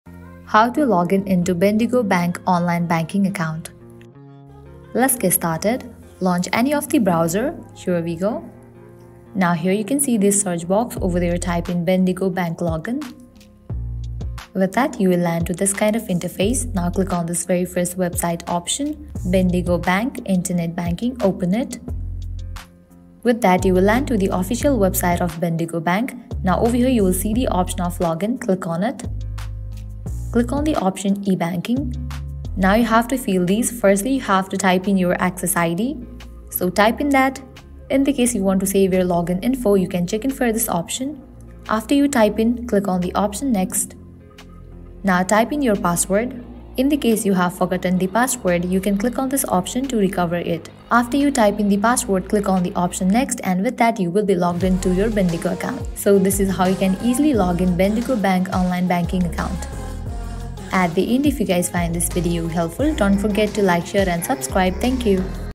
How To Login Bendigo Bank Online Banking Account (2023 GUIDE)
🏦 Ready to Access Your Bendigo Bank Online Banking Account in 2023? Here's a Step-by-Step Guide!

Logging into your Bendigo Bank online banking account is essential for managing your finances conveniently, and we're here to walk you through the process with an updated guide for 2023.

In this tutorial:
0:00 - Introduction
0:05 - How to Login Bendigo Bank Online Banking Account (2023 Guide)

Bendigo Bank offers a range of online banking services, and with our step-by-step instructions, you'll have no trouble accessing your account to check balances, pay bills, and more.

Follow our guide to log in securely, manage your finances effectively, and stay in control of your banking needs with Bendigo Bank in 2023. Get ready to take charge of your financial journey!